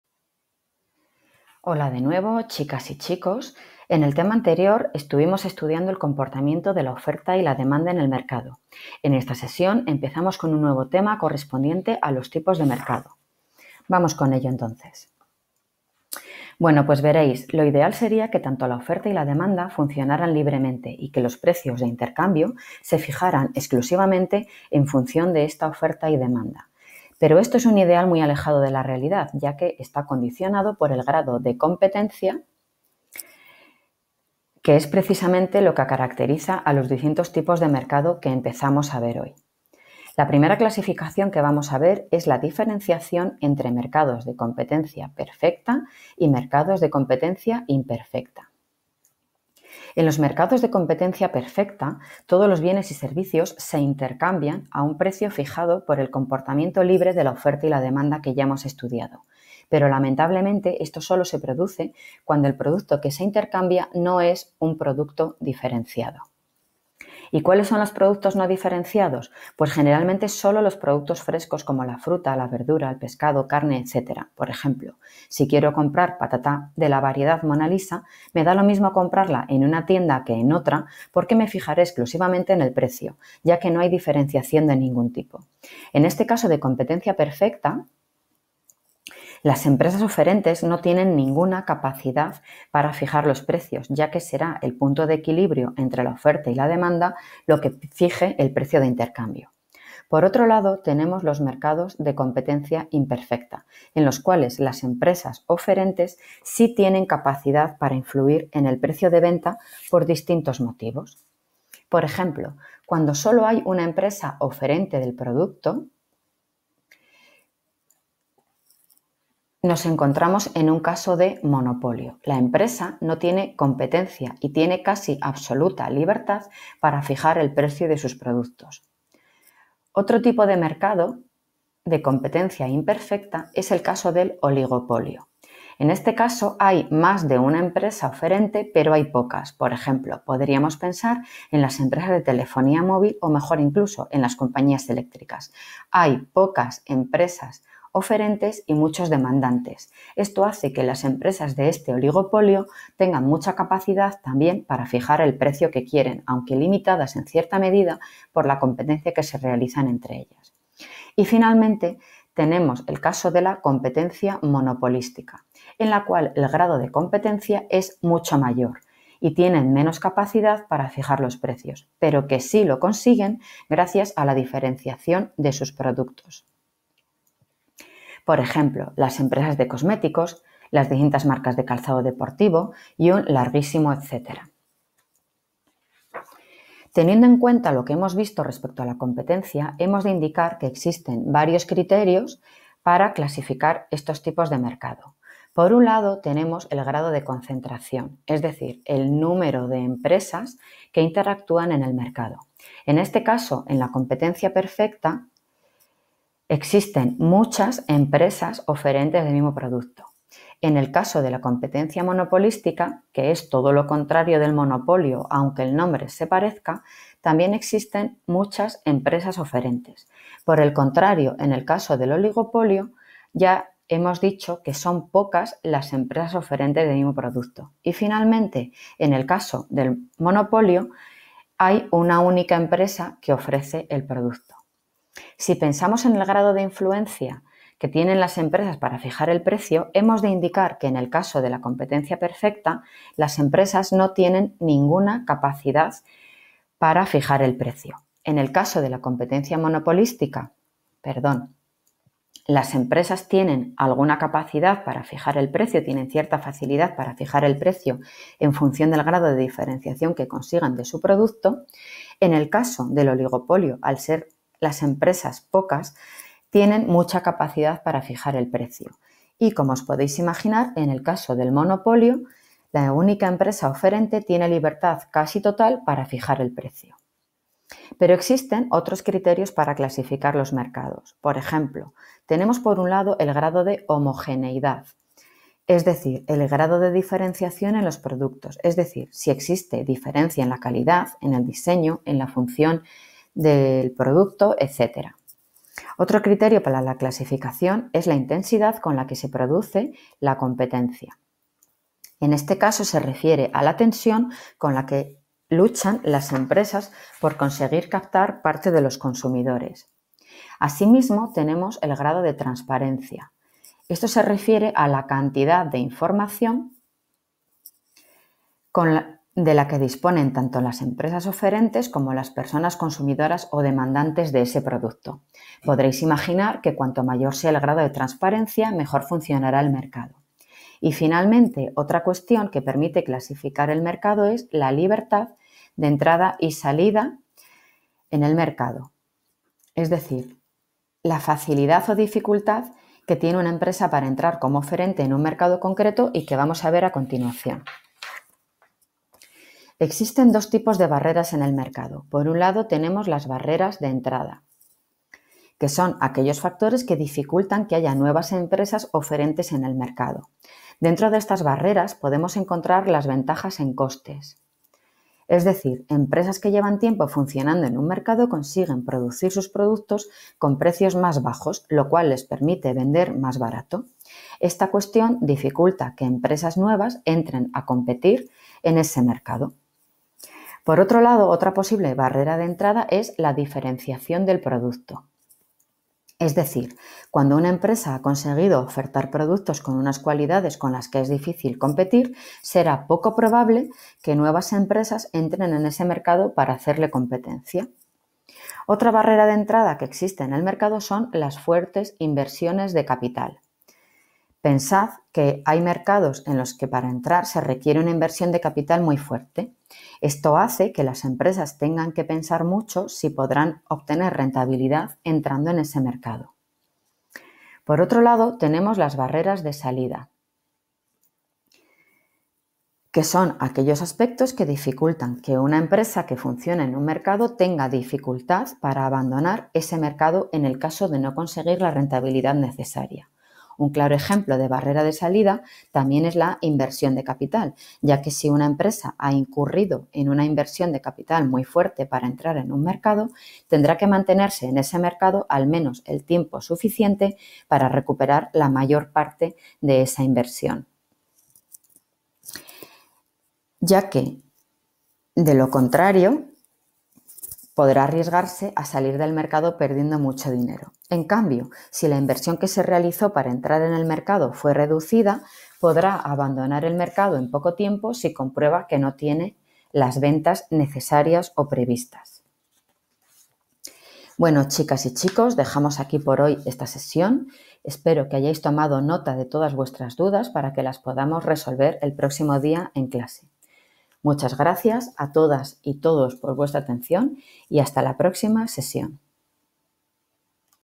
Tipos de mercado 1: Criterios de clasificación de mercados
Mercados de competencia perfecta y mercados de competencia imperfecta: Monopolio, Oligopolio y competencia Monopolística.
Distintos criterios para clasificar los mercados